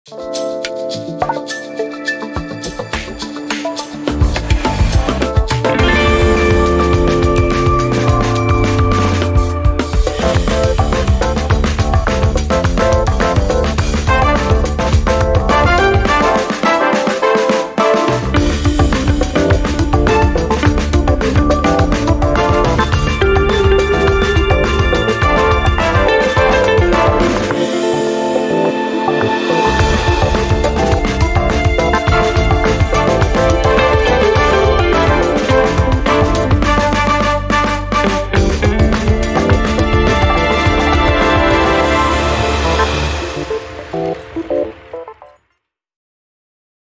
EYE CANDY
A summer awaiting visual exploration of techniques learned within the last year.

Animation by: Daniel Meszaros
Software used: Cinema 4D, SideFX Houdini, Redshift, Davinci Resolve